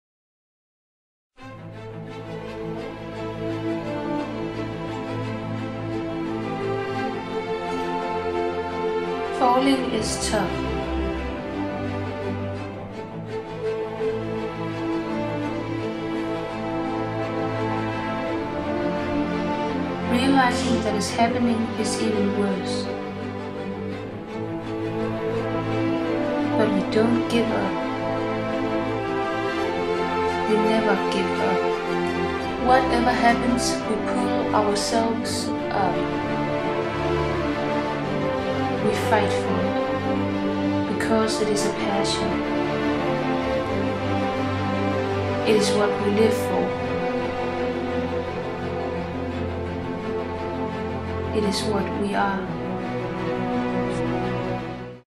Never give up!
En lille fim jeg har lavet i imovie, med inspiration fra en video på youtube :-) Jeg håber i fanger budskabet i den! Men det er for at fortælle mig selv om, hvor jeg aldrig giver Baloo op. Ligemeget hvor svært det har været, så har jeg altid forsøgt at se de lyse sider, og jeg levede højt i flere måneder på bare én positiv ridetur. Jeg elsker dig Baloo.